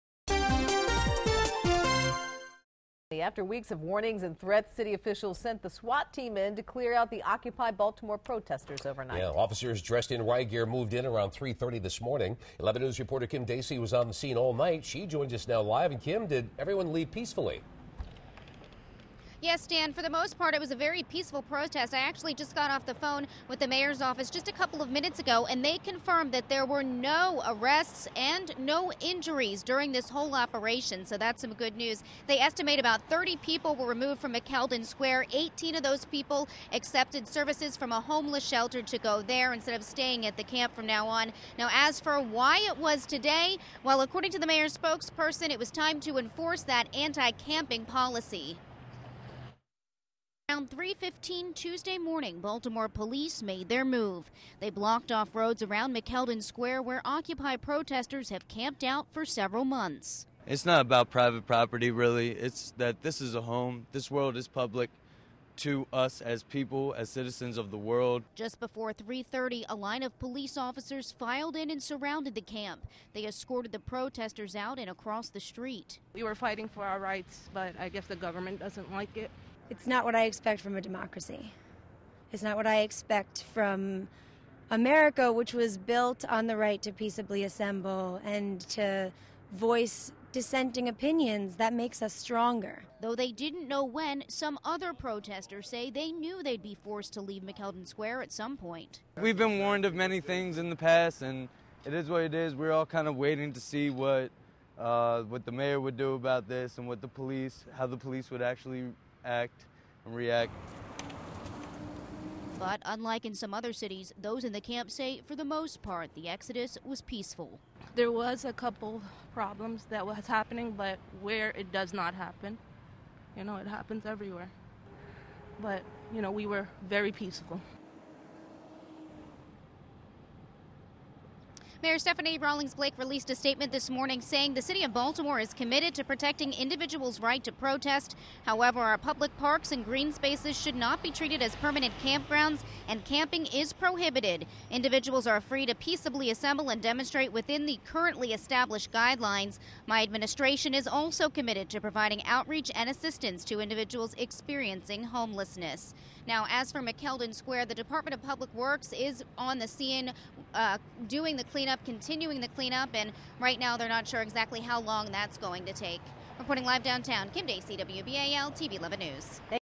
Occupy Protesters Cleared From McKeldin Square
Baltimore City police officers closed downtown streets overnight as officers in riot gear cleared McKeldin Square, the plaza where Occupy Baltimore protesters built an encampment.